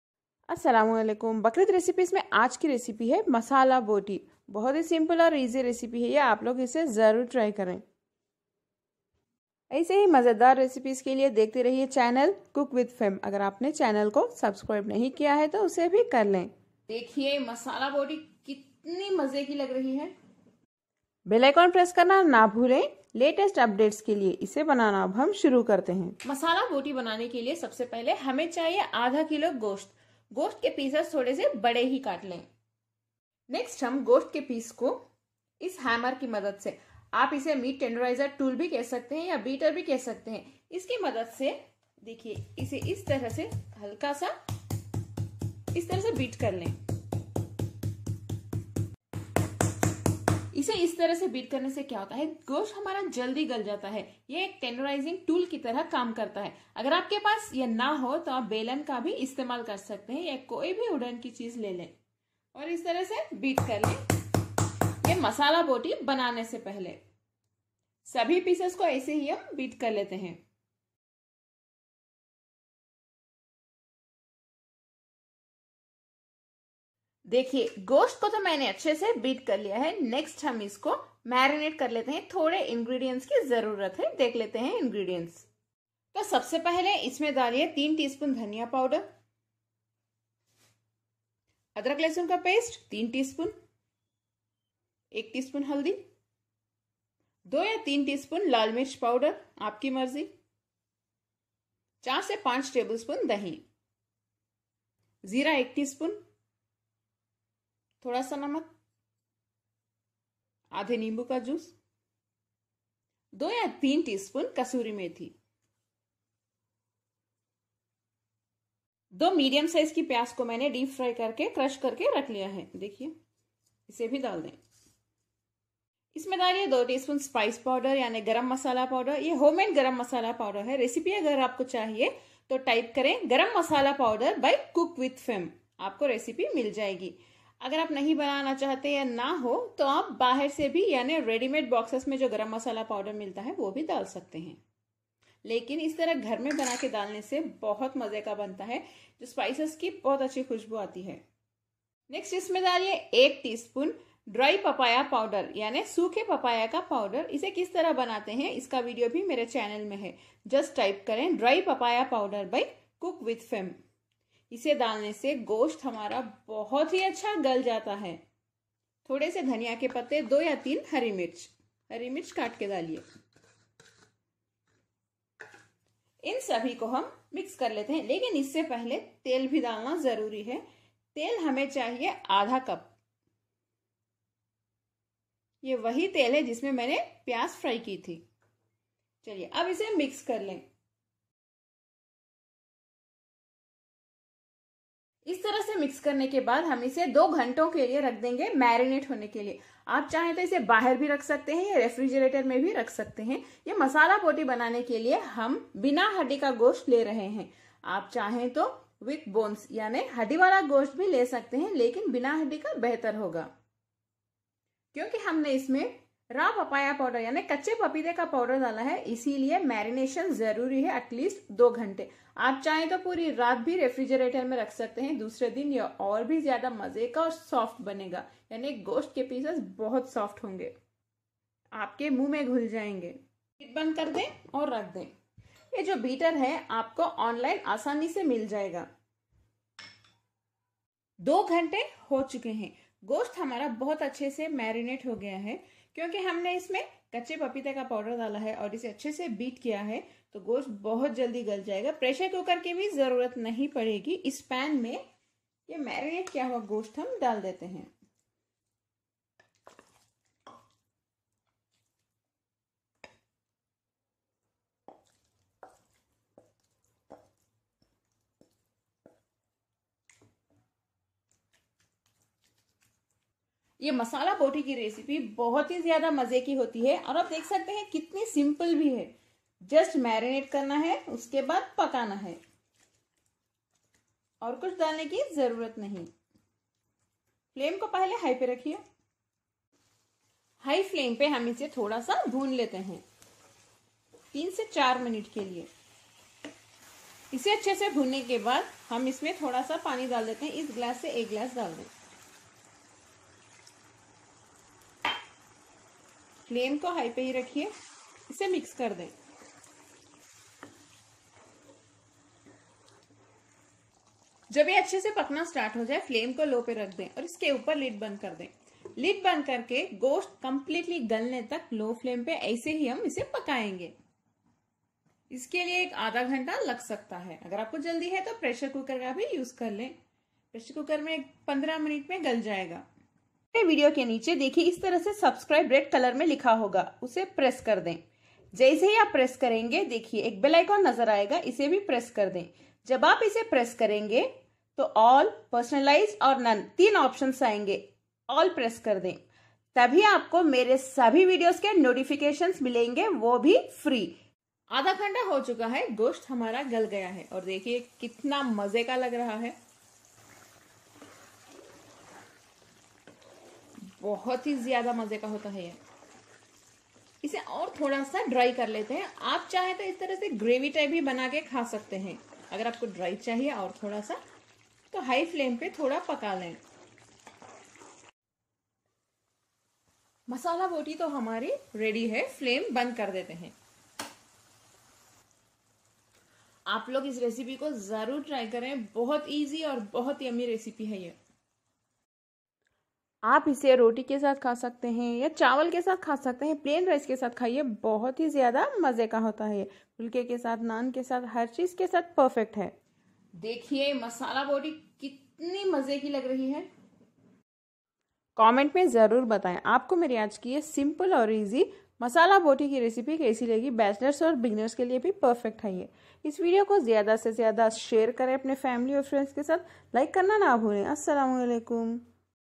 Masala Boti | Old Delhi Special Masala Boti Recipe | Bakrid special | Cook With Fem
Masala boti is an amazing mutton recipe so easy that anyone can try it, even bachelors and beginners can give it a try , finger licking mutton boti just a step away..a must try recipe

INGREDIENTS
Mutton - 1/2 kilo
Salt
Half a lemon juice
Dried fenugreek leaves - 3 teaspoon
Fried and crushed onions - 2 onions
Spice powder - 2 teaspoon
Oil - 1/2 cup
Dry papaya powder - 1 teaspoon

HERE I SHARE TASTY VEG RECIPES,DESSERTS & SNACKS